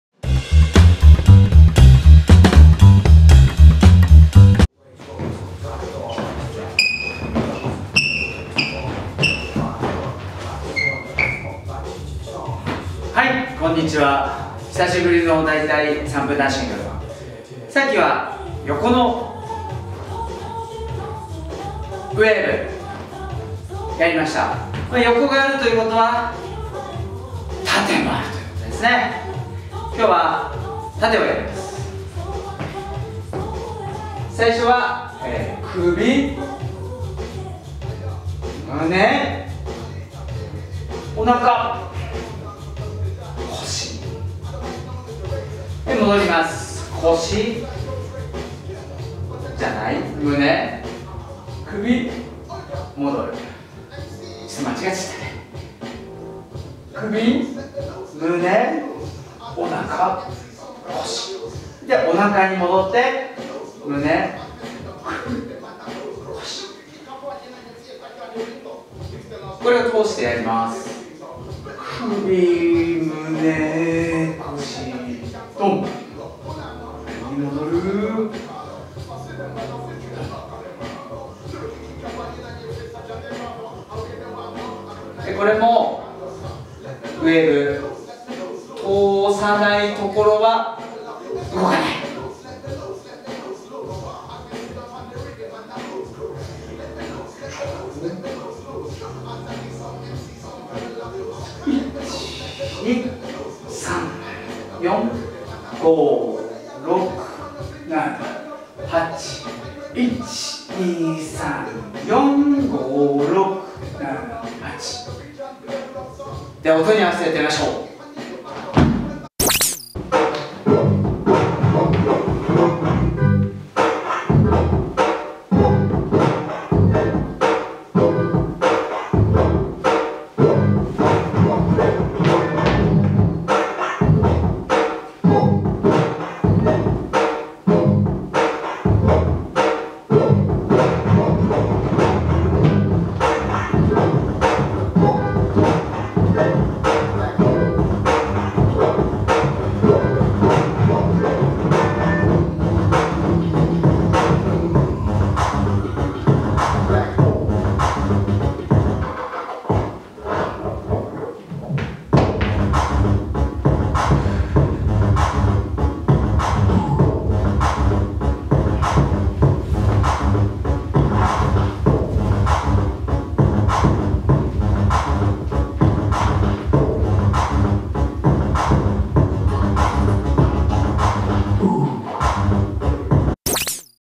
基礎⑬｜ボディウェーブ縦｜だいたい3分ダンシング♪
今回のだいたい3分ダンシングは！縦ウェーブ
身体の中を移動していくイメージでヌルヌルとやるといいです。
軟体動物になった気持ちで音に乗せてLet'sウェーブ♪